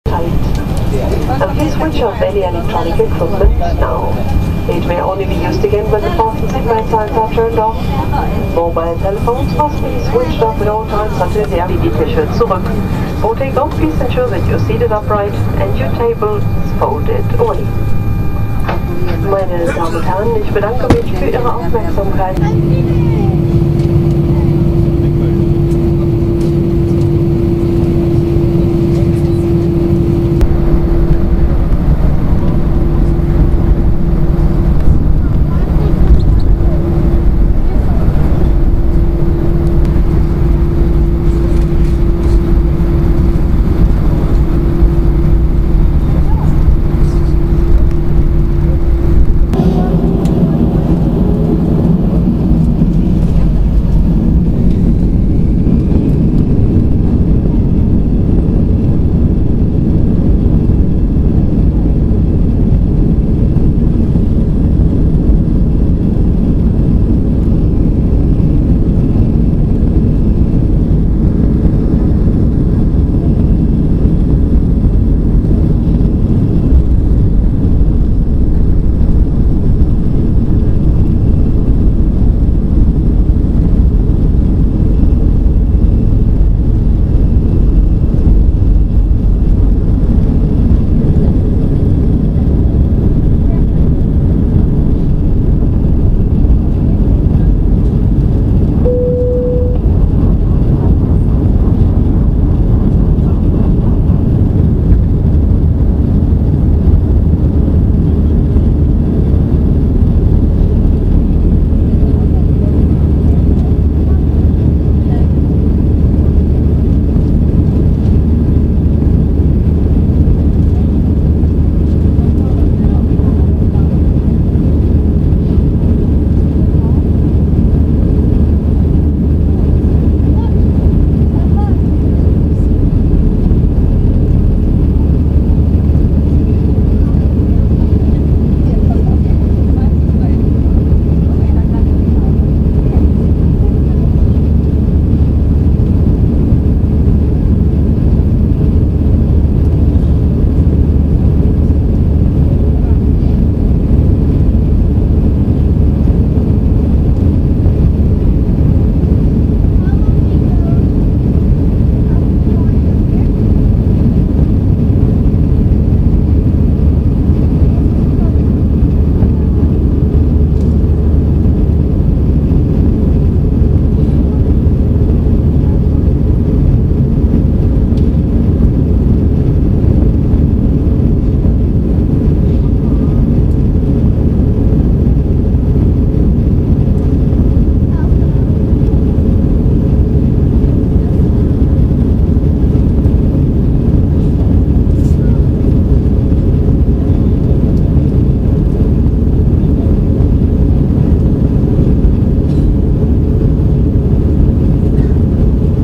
Lufthansa A320 takeoff in Frankfurt [1080p full HD]
___________________________INFOS____________________________
the flight was at 1/8/2011.
destionation: Paris CDG